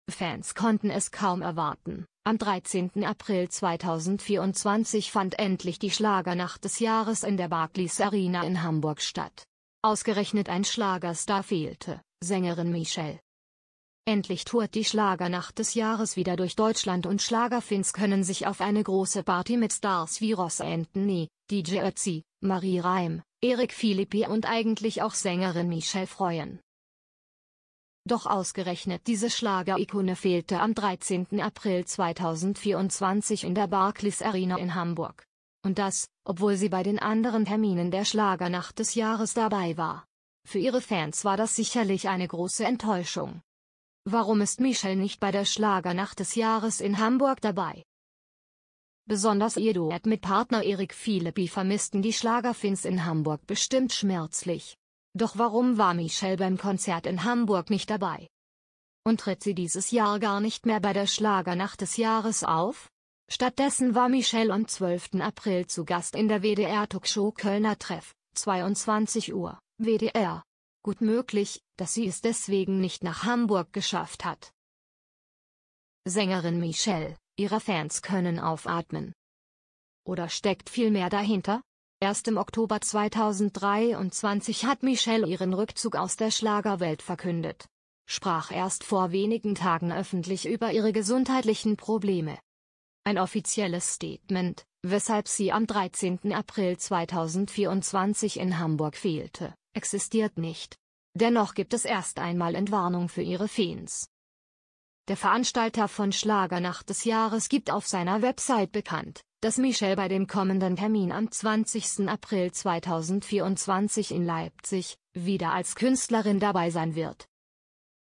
Michelle Überraschung! “Schlagernacht des Jahres” plötzlich ohne sie
Fans konnten es kaum erwarten: Am 13. April 2024 fand endlich die “Schlagernacht des Jahres” in der Barclays Arena in Hamburg statt. Ausgerechnet ein Schlagerstar fehlte: Sängerin Michelle!